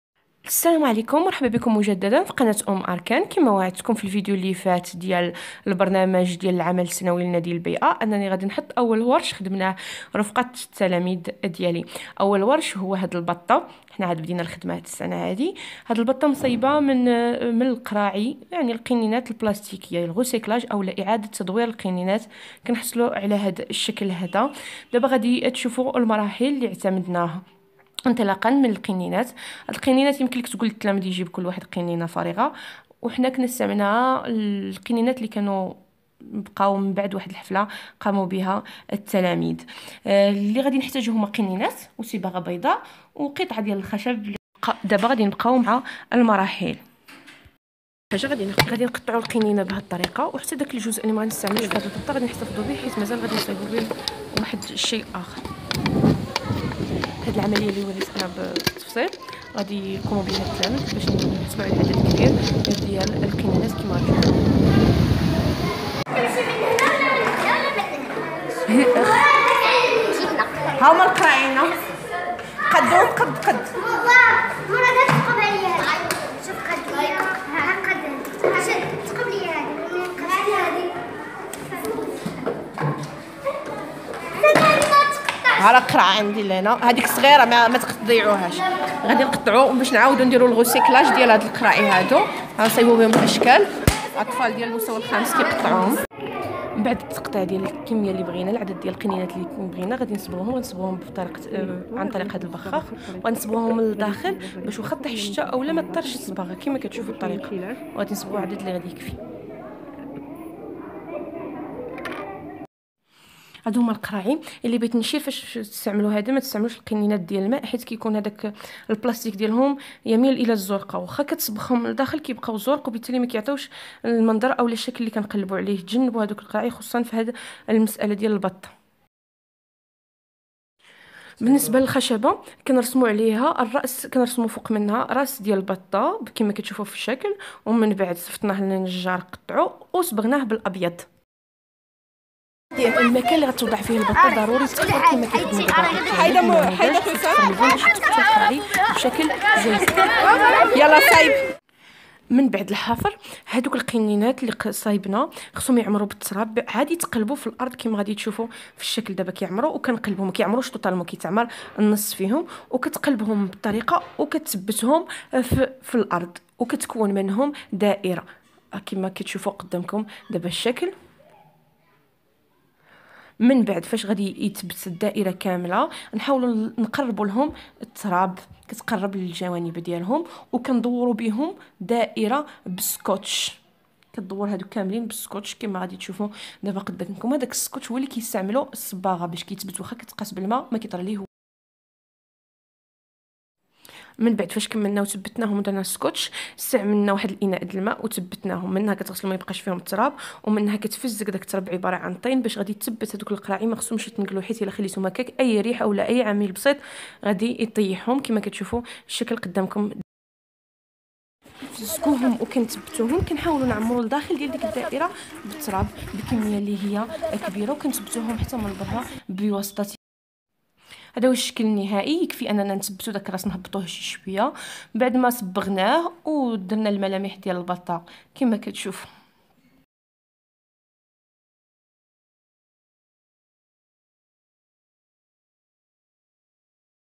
ورش تدوير النفايات في اطار عمل النادي البيئي
تدوير القنينات البلاستيكية للحصول على بطة من اجل تحسين جمالية المدرسة الوسائل المستعملة:
قنينات بلاستيكية
صباغة بيضاء
قطعة خشب